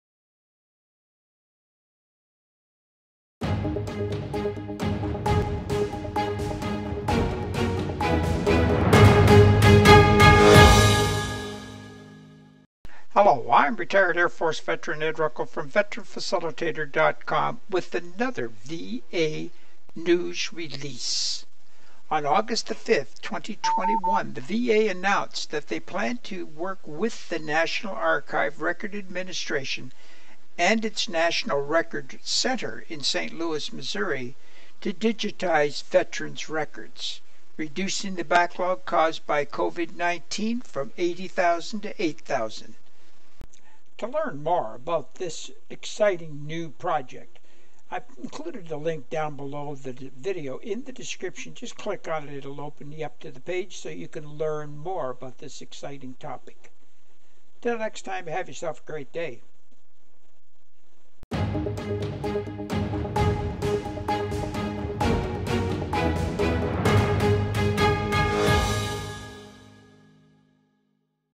veteran facilitator newsroom|VA digitized Digital Veterans Records
veteran facilitator newsroom|VA digitized Digital Veterans Records. Digitizing Veterans records will reduce the backlog of open claims from 80,000 records to 8,000 records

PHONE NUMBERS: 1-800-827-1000